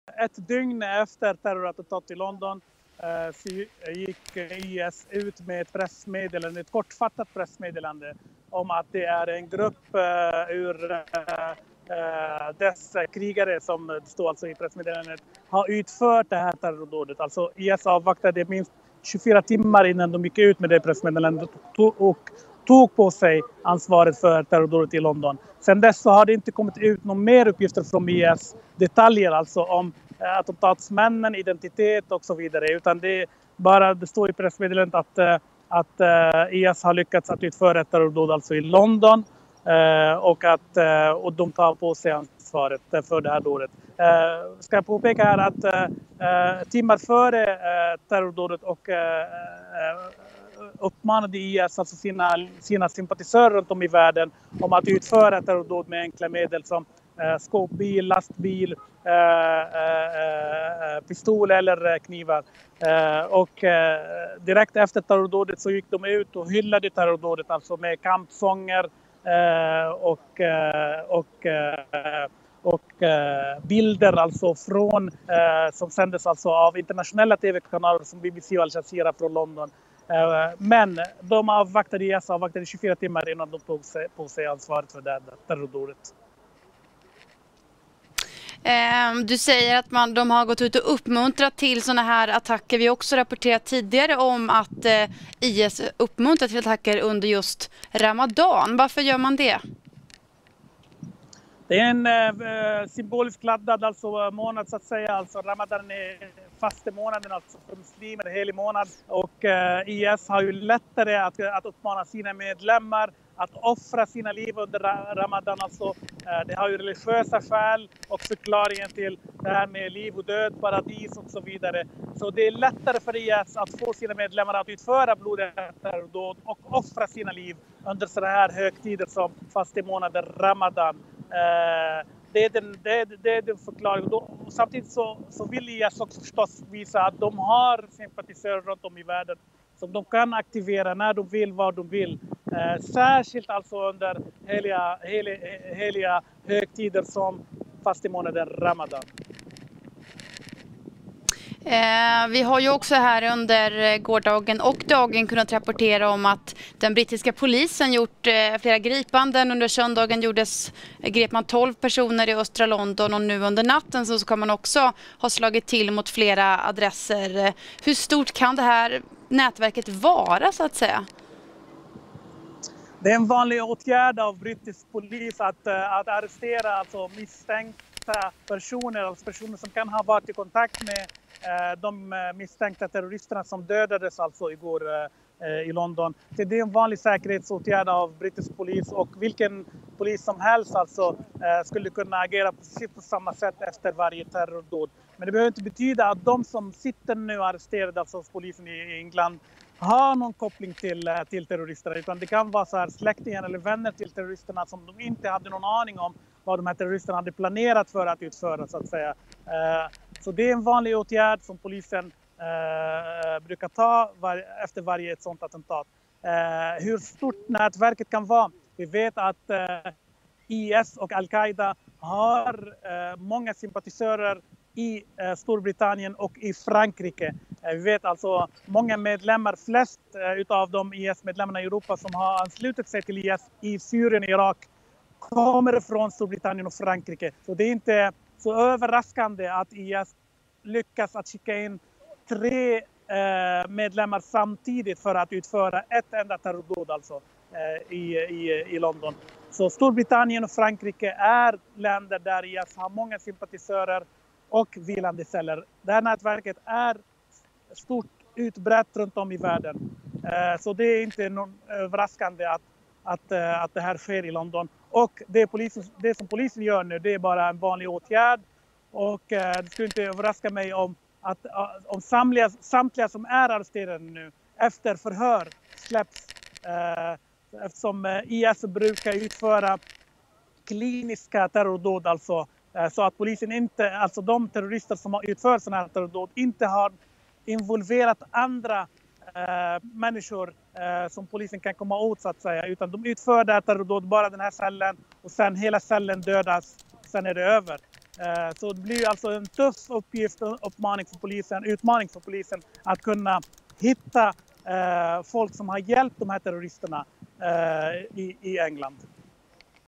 Terrorgruppen IS tar på sig ansvaret för terrorattacken i London
Terrorgruppen IS tar på sig ansvaret för terrorattacken i London - Expressens Mellanösternkorrespondent Kassem Hamadé analyserar